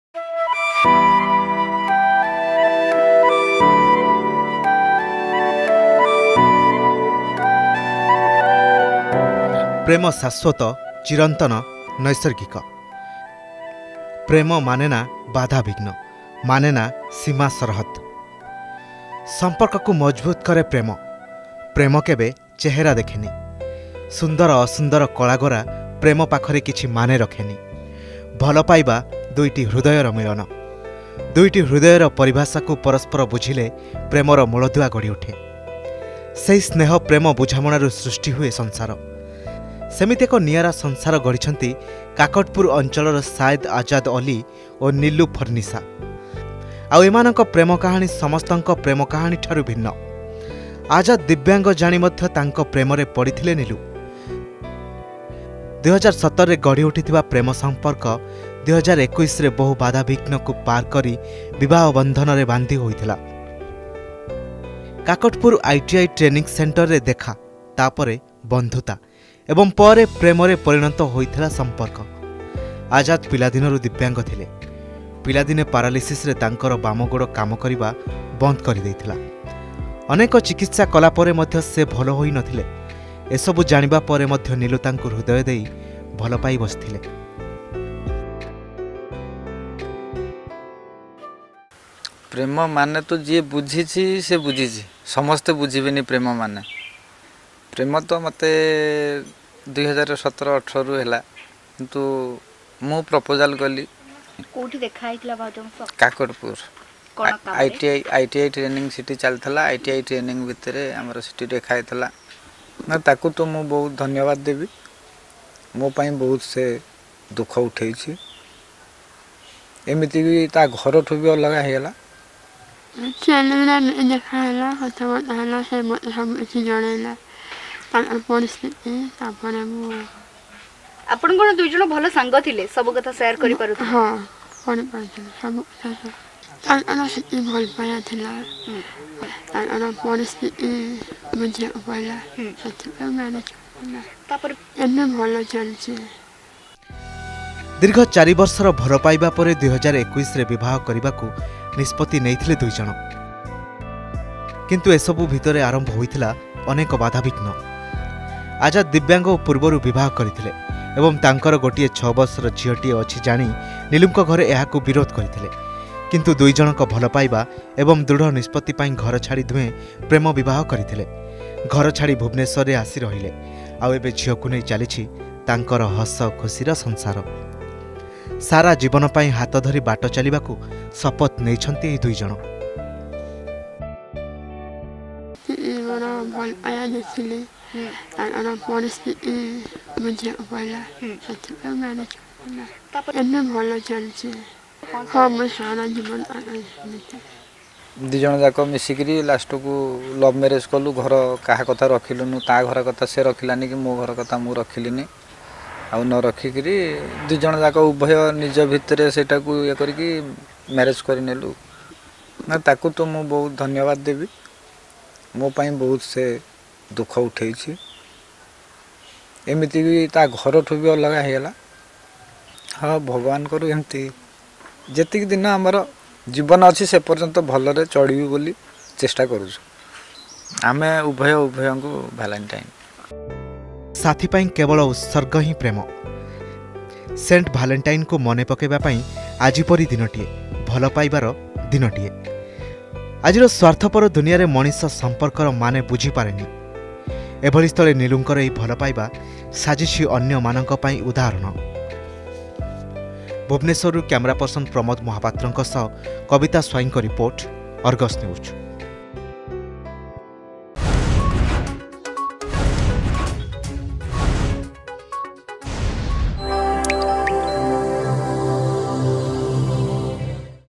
Love knows no Boundaries, A Special Love Story of a Divyang
Watch the Special Love Story of Divyang Sayad Azad Ali and his wife Nilu Farnisha on Valentine's Day.